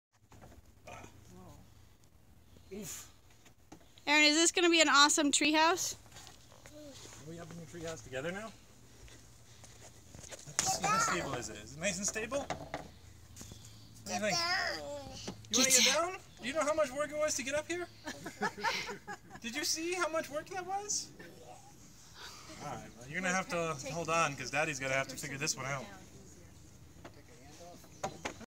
Aeryn's First Treehouse Ascent
Climbing up into the treehouse before it's quite ready.